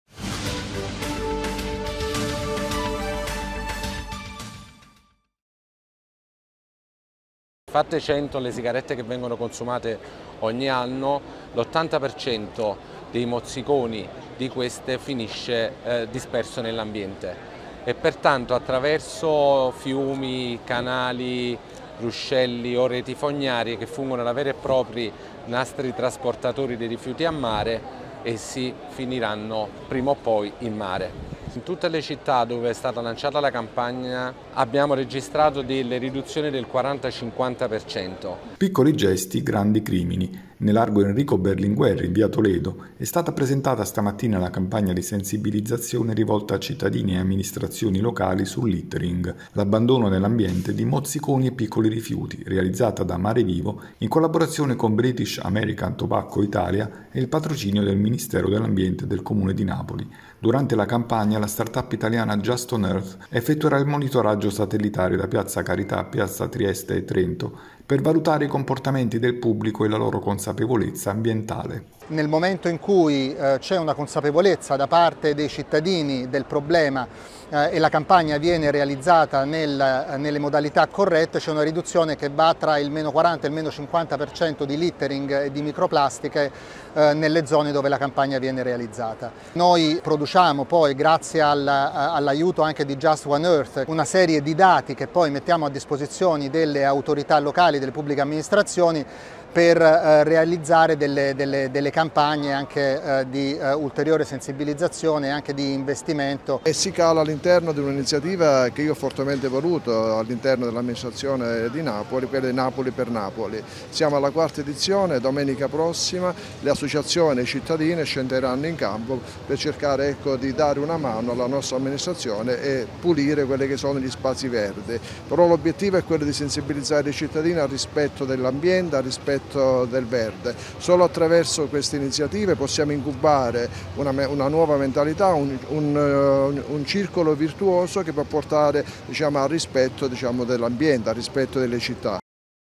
“Piccoli gesti, grandi crimini”, contro l’abbandono di mozziconi e piccoli rifiuti
Arriva a Napoli la campagna di sensibilizzazione sul “littering” realizzata da Marevivo in collaborazione con British American Tobacco Italia e il patrocinio del Ministero dell’Ambiente e del Comune di Napoli.